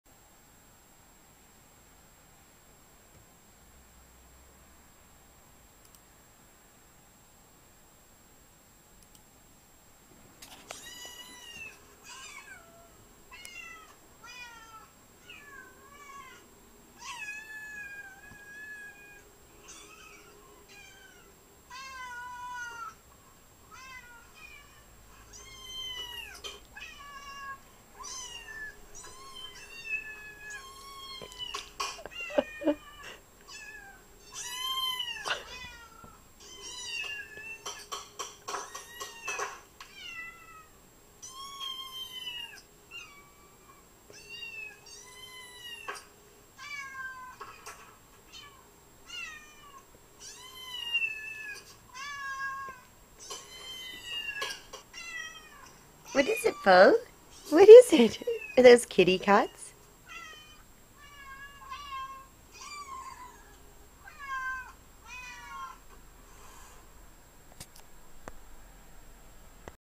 Puppy confused by youtube video
Foe is wondering where the cats are :)  No idea how to rotate the video, sorry.